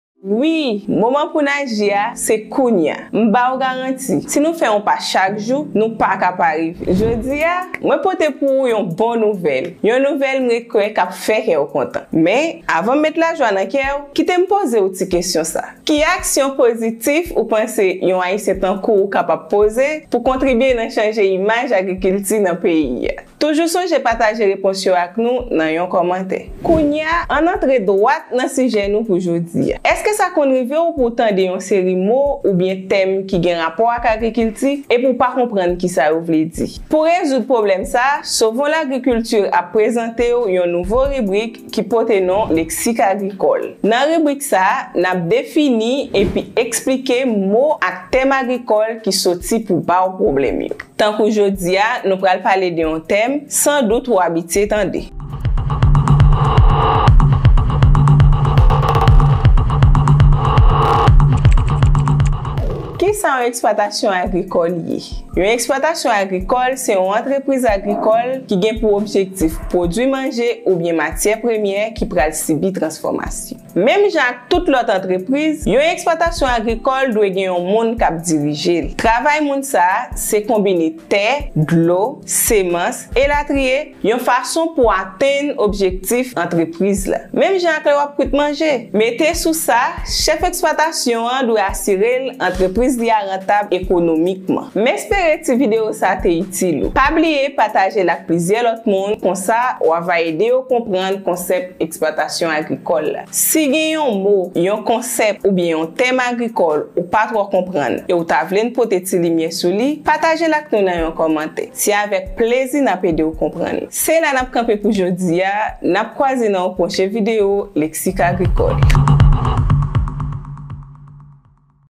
Eksplwatasyon agrikòl #Leksik agrikòl #1
Nan kad ribrik Leksik agrikòl , n ap prezante yon tèm enpòtan nan mond agrikòl la. Tèm sa se baz mond agrikòl la.

 Kisa yon eksplwatasyon agrikòl ye?

Nouvelle video chaque weekend...
Ensemble œuvrons pour une agriculture durable dans le monde.